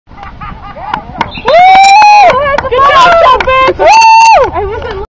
Toilet Bowl 2008: CMU Trumpets vs CMU Trombones 1
Toilet Bowl 2008: CMU Trumpets vs CMU Trombones, Final score Trumpets: 28 Trombones: 7